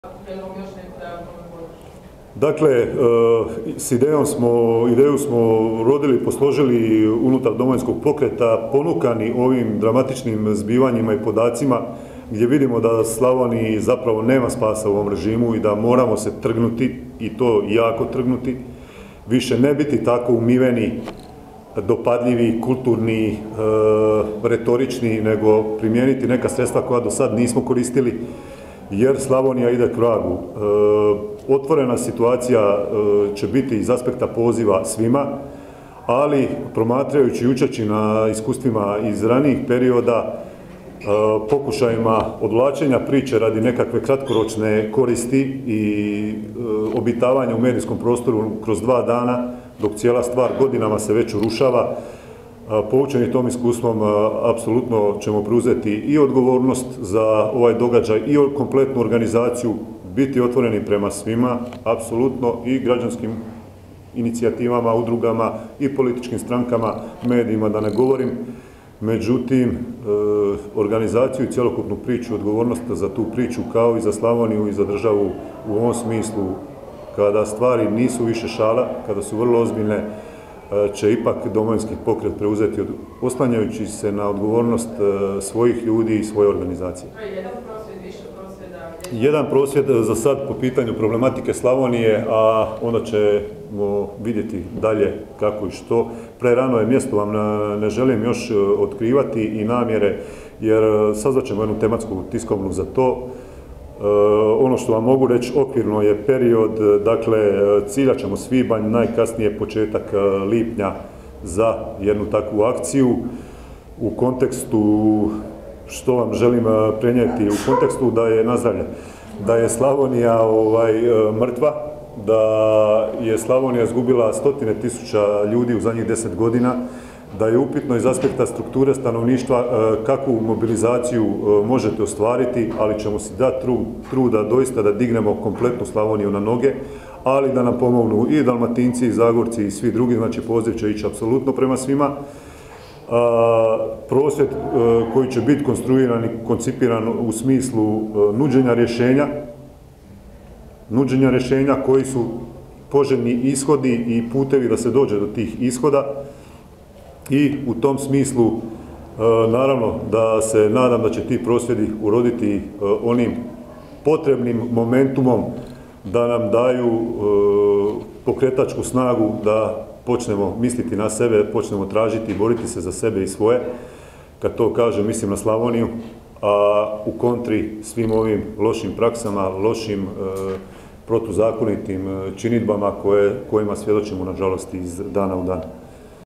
Penava: "Domovinski pokret preuzima odgovornost za prosvjed"
Na konferenciji za medije odrzanoj u Saboru o aktualnim temema govorio je zastupnik Ivan Penava.